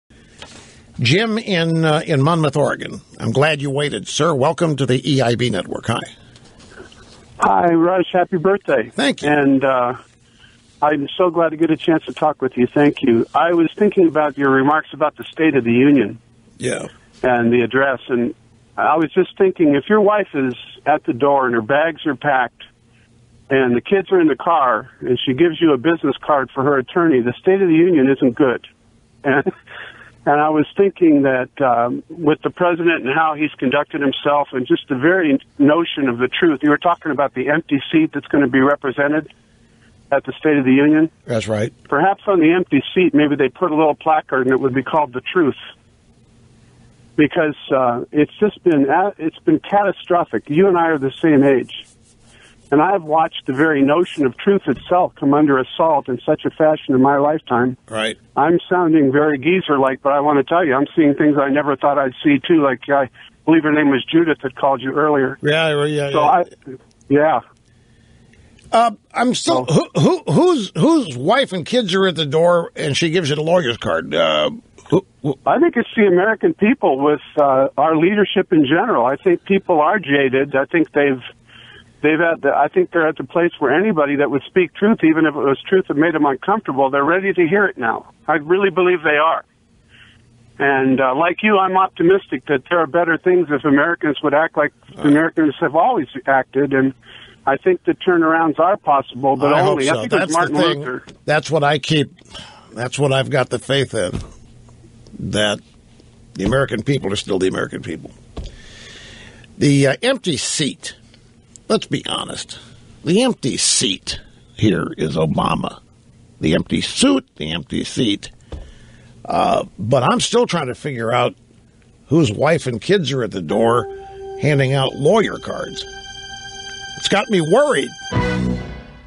Rush Limbaugh on being a "general optimist"
Rush Limbaugh (on 1/12/16, his 65th birthday): "I do believe that, push come to shove, a majority of Americans will come to their senses or are in the process of doing it right now, and that there will be a tipping point at some point."